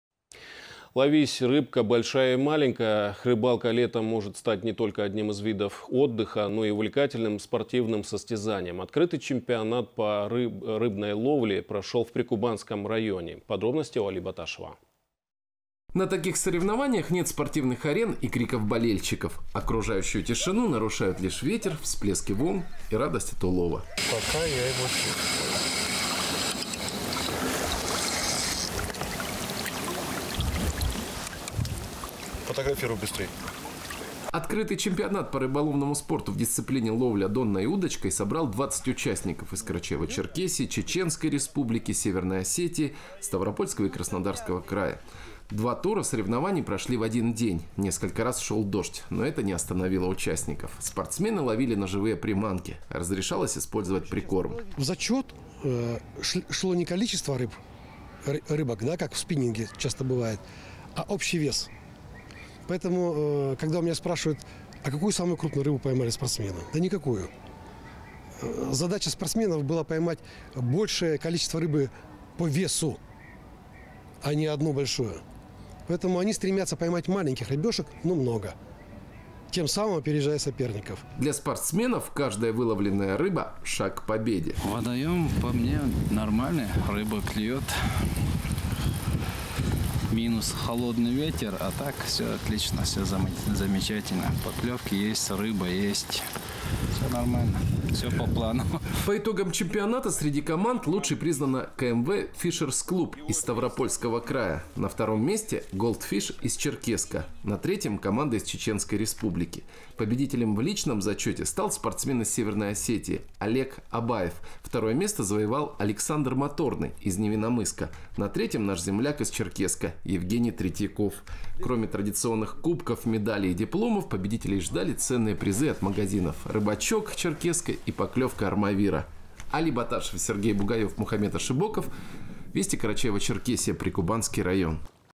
Открытый чемпионат по рыбной ловле прошел в Прикубанском районе
Ловись, рыбка, большая и маленькая. Рыбалка летом может стать не только одним из видов отдыха, но и увлекательным спортивным состязанием